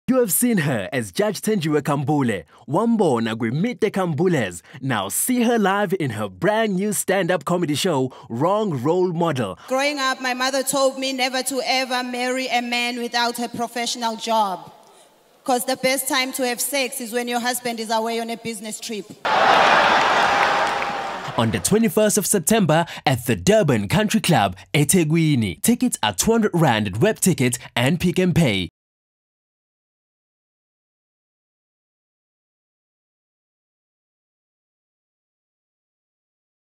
Thenjiwe comedy show promo
My new show Wrong Role Model
 Durban  21 September
London (UK) 19 October